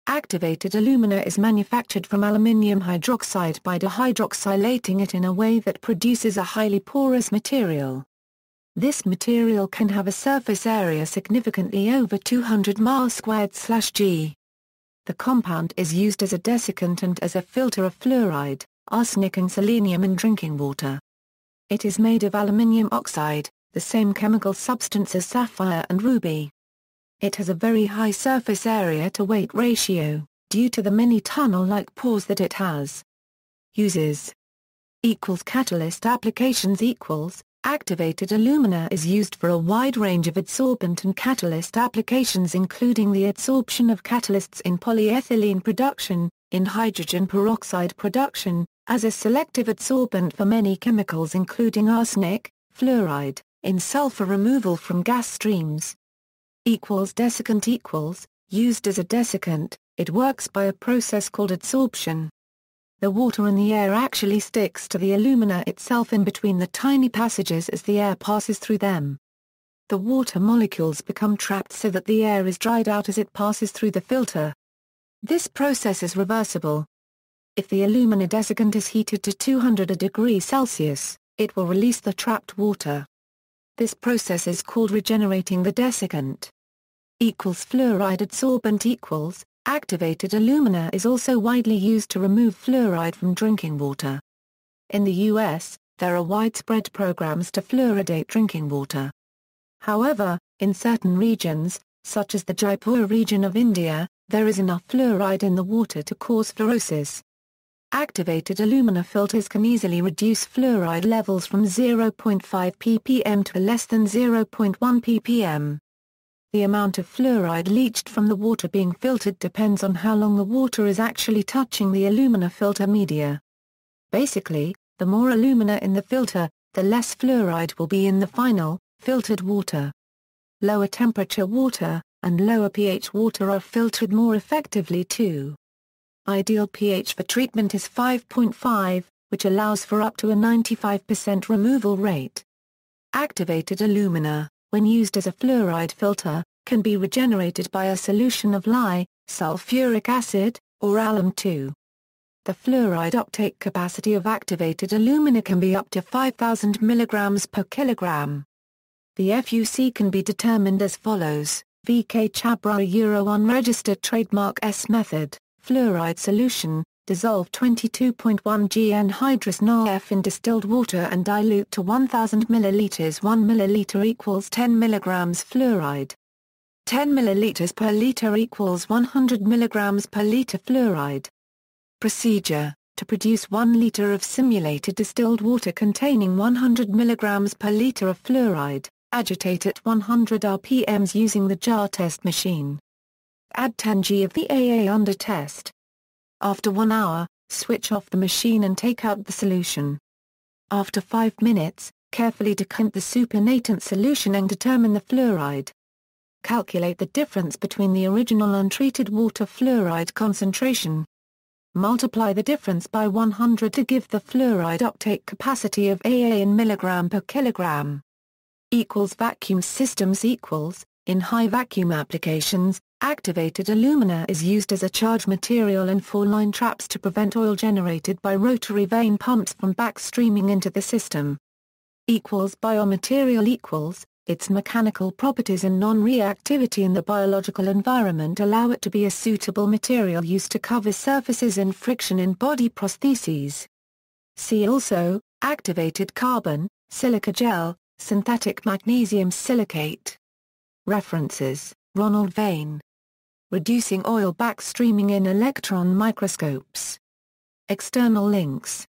Activated alumina
Activated alumina is manufactured from aluminium hydroxide by dehydroxylating it in a way that produces a highly porous material; this material can have a surface area significantly over 200 m²/g. The compound is used as a desiccant and as a filter of fluoride, arsenic and selenium in drinking water. It is made of aluminium oxide, the same chemical substance as sapphire and ruby. It has a very high surface-area-to-weight ratio, due to the many "tunnel like" pores that it has.